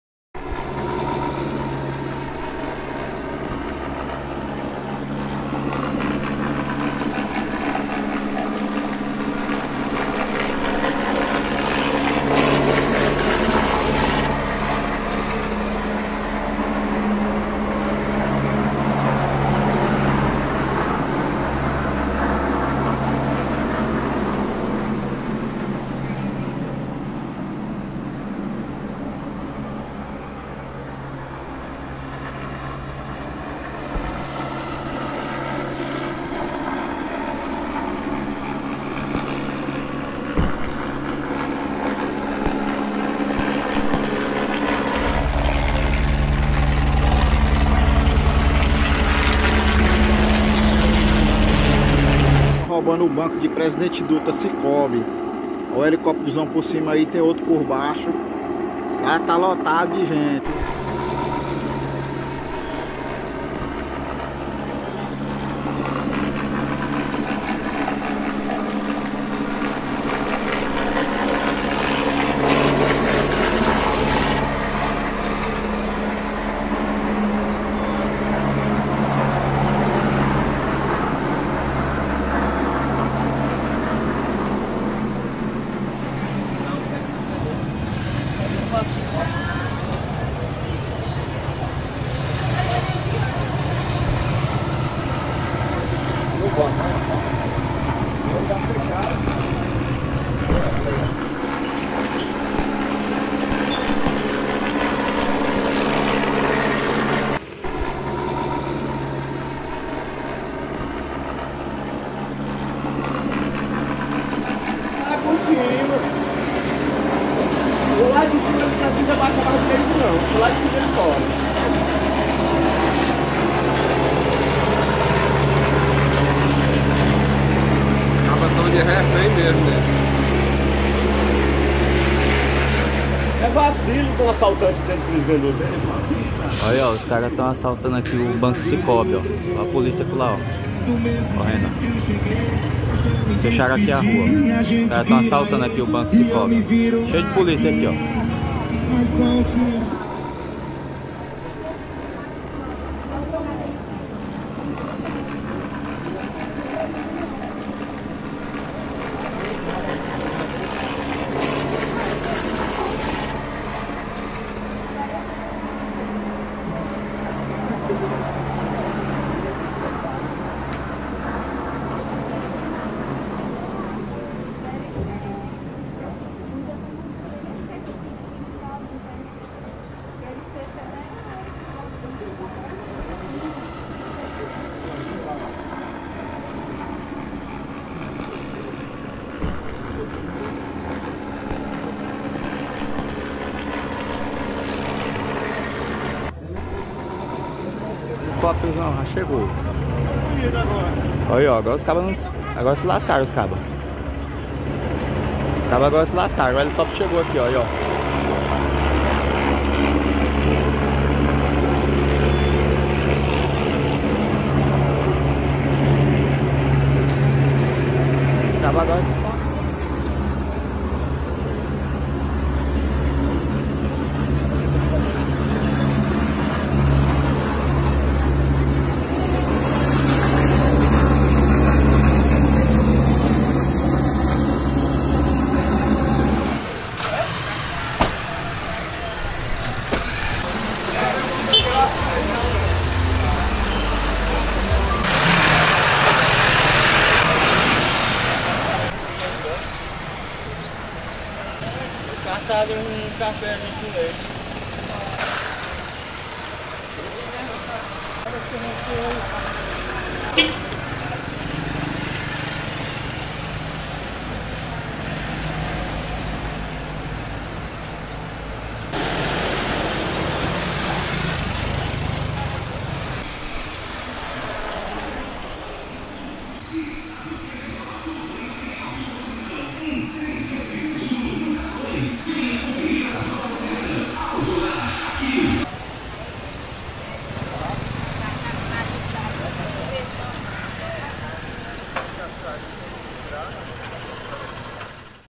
FORÇAS POLÍCIAIS FRUSTRAM TENTATIVA DE ASSALTO À AGÊNCIA BANCÁRIA EM PRESIDENTE DUTRA
A polícia da cidade de Presidente Dutra, Maranhão, conseguiu evitar um assalto ao banco Sicoob. Assim que a gerente viu alguns meliantes tentando entrar no banco ela conseguiu avisar a polícia que imediatamente chegou no local, atiradores de elite e até helicóptero foi utilizado na ação que terminou com ninguém ferido.

Os assaltantes fugiram do local e levaram apenas a arma do vigilante.

Segundo o delegado César ferro, os assaltantes não são de Presidente Dutra e os agentes já estão em campo no intuito de localizá-los.

Copyright@2022

Compartilhe !!!

REPÓRTER: ADONIAS SOARES
IMAGENS | REPÓRTER CINEMATOGRÁFICO: ISRAEL DELAMARK E CARLOS MAGNO
EDIÇÃO DE IMAGENS: ISRAEL DELAMARK

2022